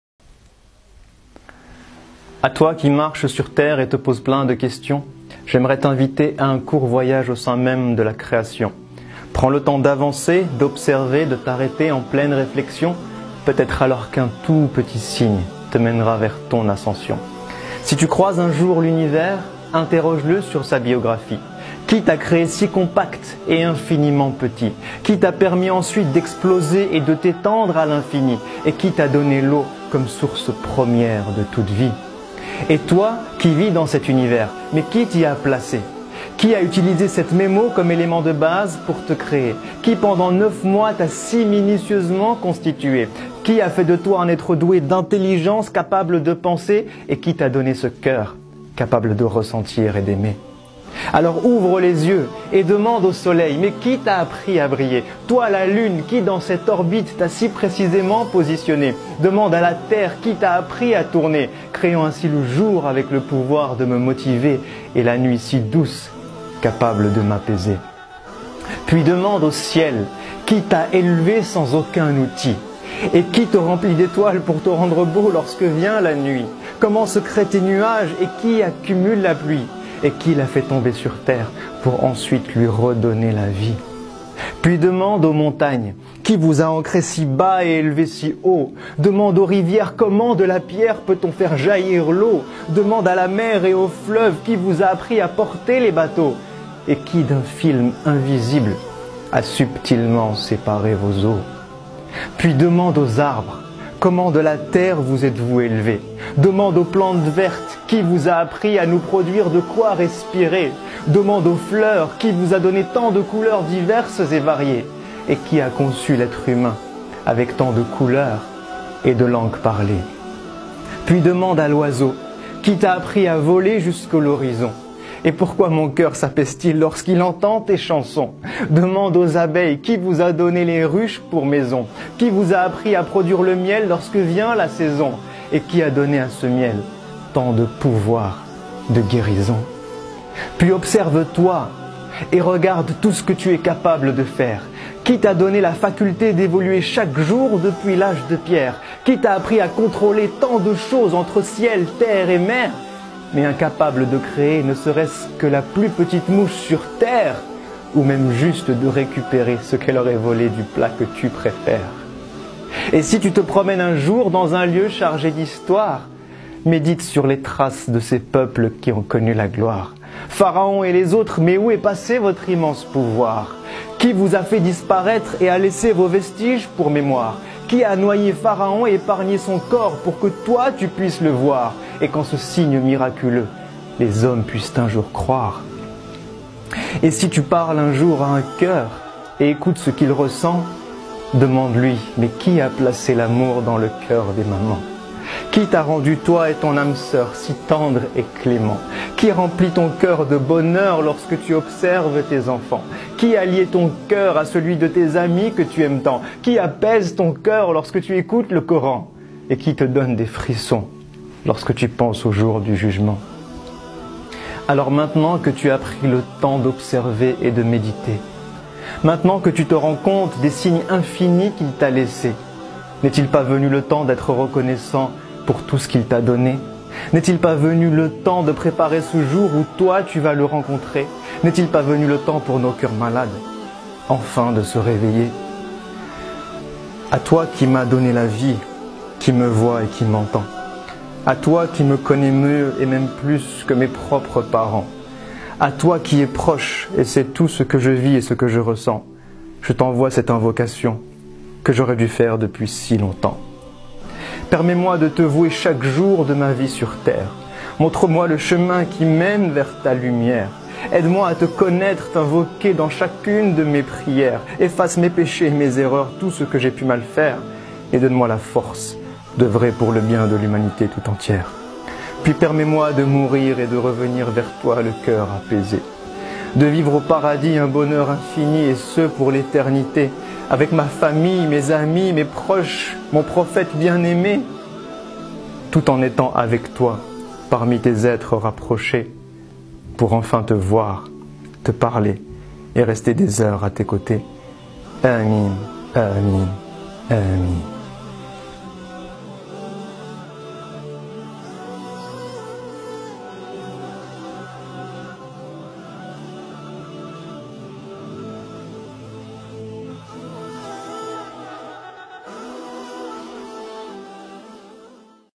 Savoir qui contrôle tout
Savoir qui contrôle toutCette vidéo contient
                              des explications Sur terre, soleil, lune et ciel
                             A propos de la vie et de tout dans ce monde
                                         Dieu contrôle tout et il est capable de
                               Écoutez la vidéo et vous comprendrez tout